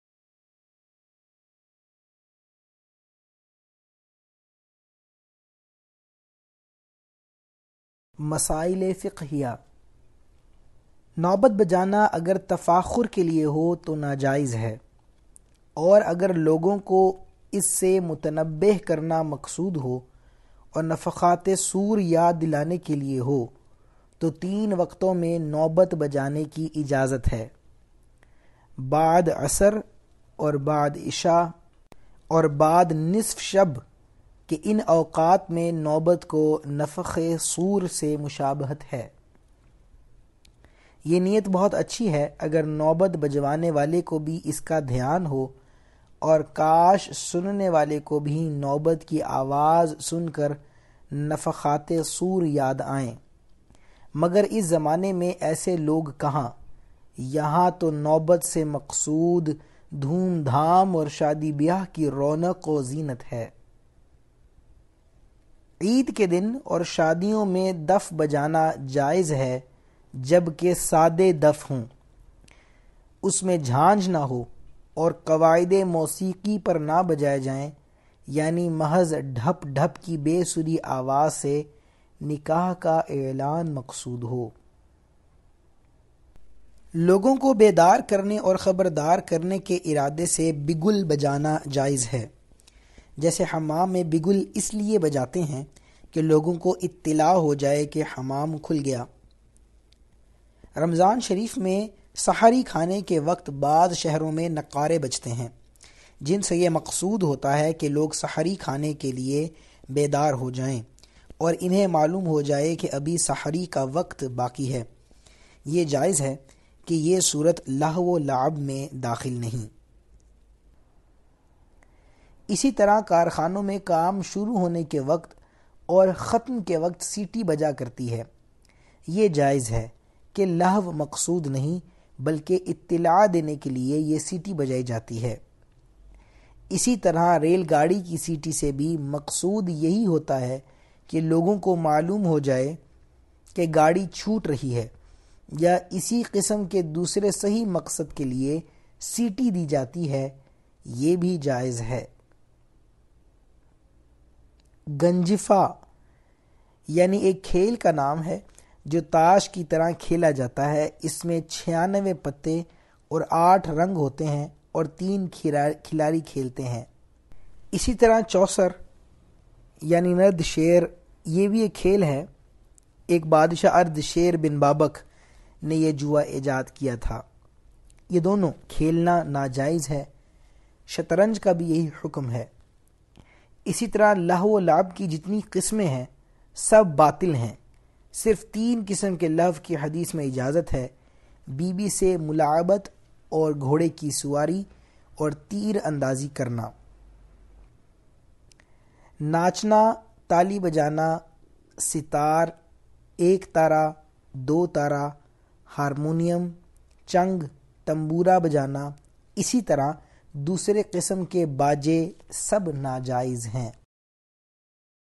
Dars e Bahare Shariat Clip-109 By Hazrat Maulana Salman Raza Qadri Owaisi Sahab.
_مسائل فقھیہ_

• نوبت، دف، بگل اور سیٹی بجانے کا حکم
• گنجفہ، چوسر، شطرنج کھیلنا ناجائز ہے
• ناچنا، تالی بجانا، ستار، ایک تارہ، دوتارہ، ہارمونیم، چنگ، طنبورہ اور باجا بجانا ناجائز ہے

*Bahar e Shariat, Clip 109*
_Masayil e Fiqhiya_

• Nobat, daf, bagal aur seeti bajane ka hukm
• Ganjifa, chausar, shatranj khelna naa jayiz hay
• Nachna, tali bajana, sitaar, aik tara, do tara, harmonium, chang, tanboora aur baja bajana naa jayiz hay